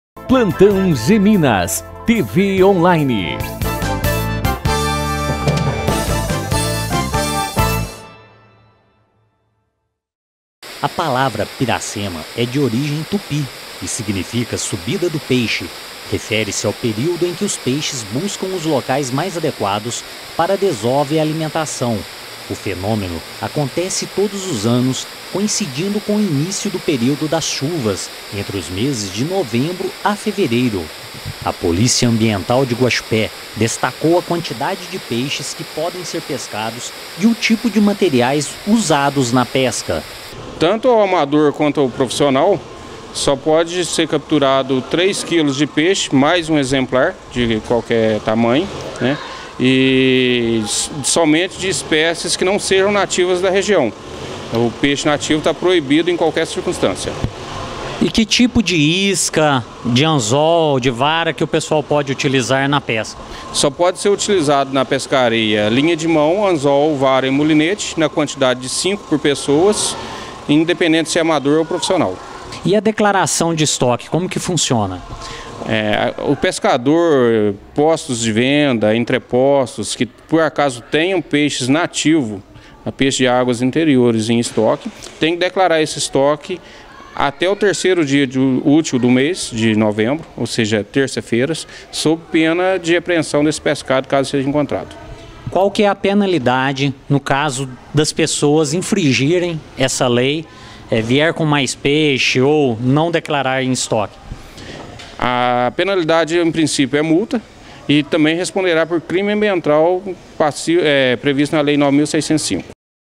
Piracema período previsto entre 1 de novembro a 28 de fevereiro de 2014
Piracema é o período de desova dos peixes que ocorre entre os meses de outubro a março. Os peixes que migram para reprodução precisam nadar contra a correnteza em uma subida árdua até as cabeceiras dos rios, para se reproduzirem. Acompanhem